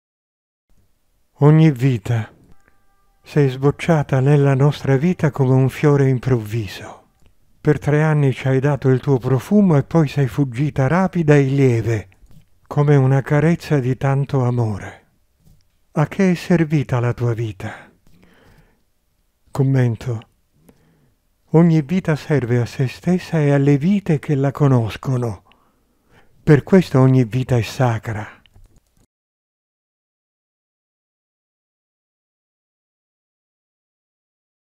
Evolvenza Ogni vita
The "photograms", the experiences, which have slightly expanded our consciousness, are called by the masters of the Cerchio firenze 77 "feel side". While experiences that broaden consciousness are called "righteous feel".